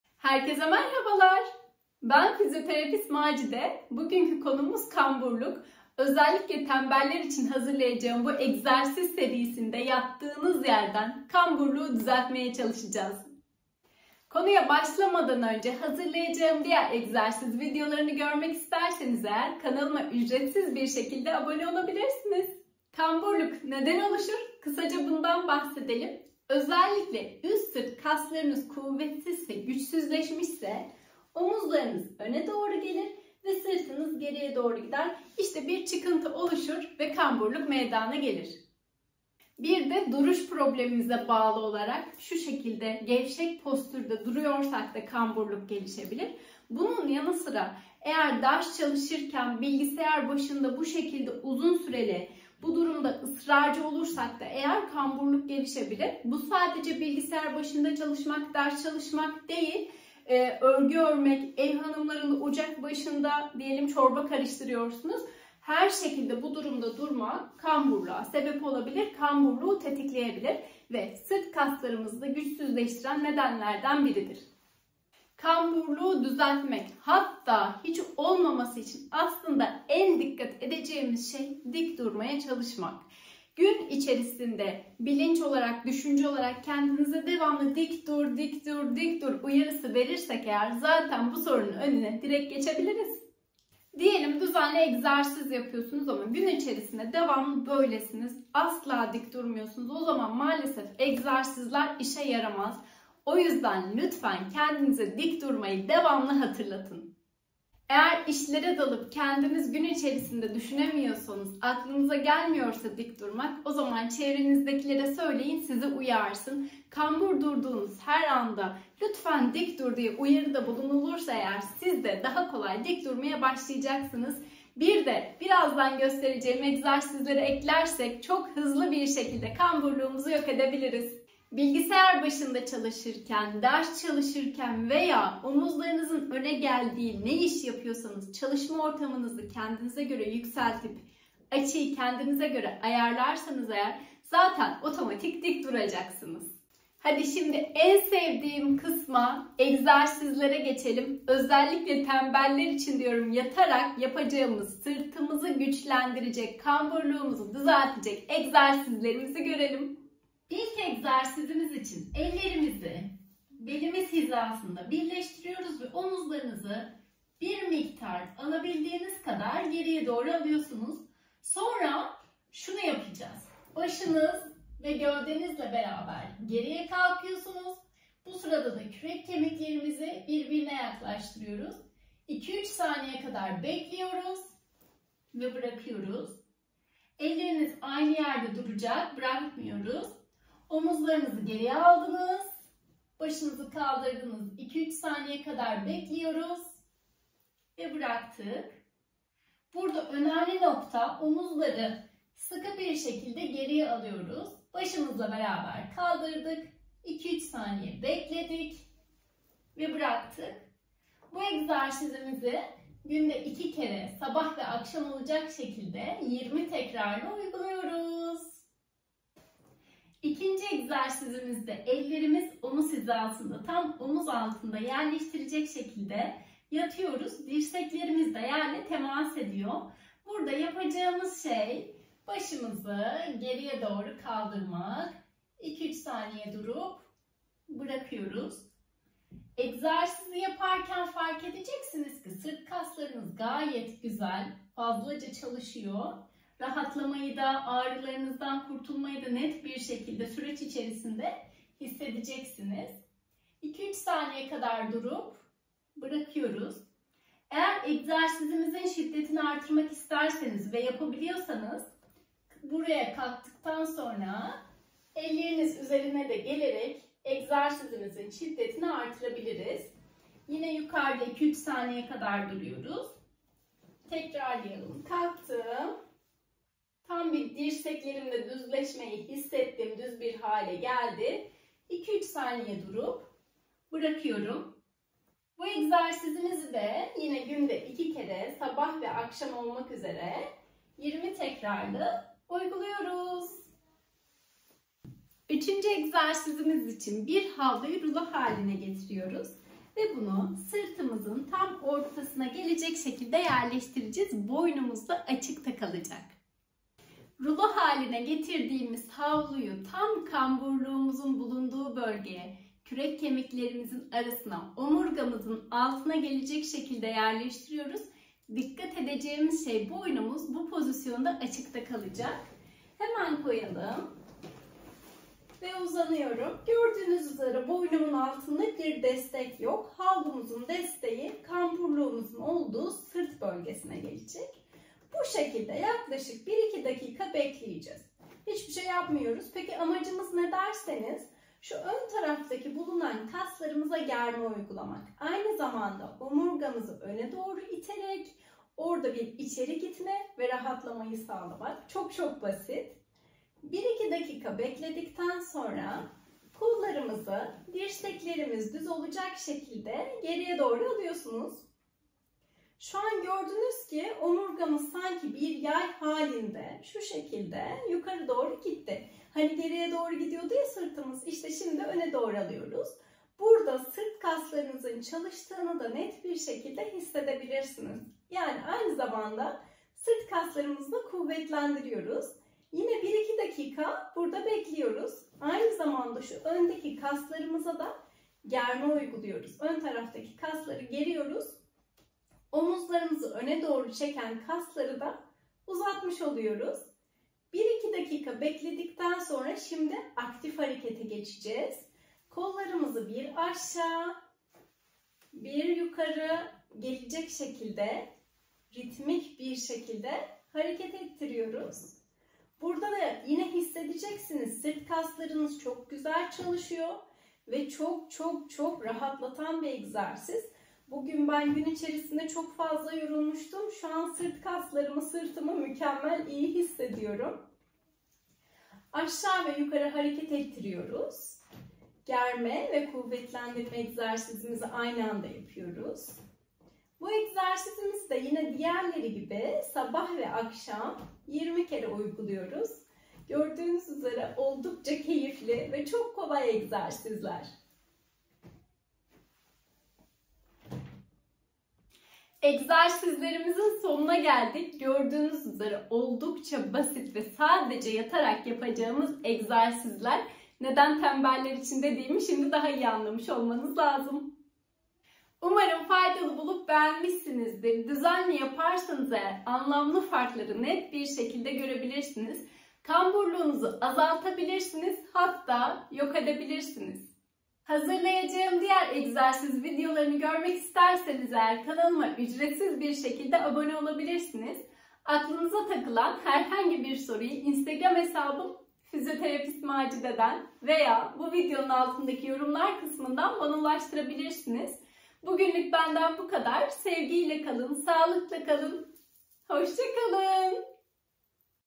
KAMBURLUĞU YATARAK DÜZELT⭐️ ÖNDE DURAN OMUZLAR / SIRT AĞRISI EGZERSİZLERİ
☘️ KAMBURLUK bir çok kişinin sorunu

📢 ÖNDE DURAN OMUZLAR da bu soruna eşlik eder. Önüne geçilmezse ilerlemeye devam edere

📌 Bu egzersizler KAMBURLUK için çoook etkili egzersizlerdir ✅

☘️ Günde 2 kere 20 tekrar yapabiliriz ✅

🌼 Faydalı bulduysanız beğenmeyi ve kanalıma anone olmayı unutmayın 🤍

🌸 Başka hangi problemlerde egzersiz görmek istersiniz yorumlarda buluşalım 😉

💚 Sağlıkla kalın 💚